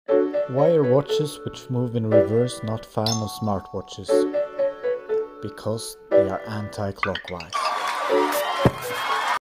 My watch is anti-clockwise 😓
Why can't we all get along within the small niché of watches. It shouldn't matter if you like modern or vintage, quartz or mechanical, microbrand or huge company. We all share the same illness. Watches ❤️⌚

Those idiots who strap tiny computers to their wrists and call it a watch can burn though 😂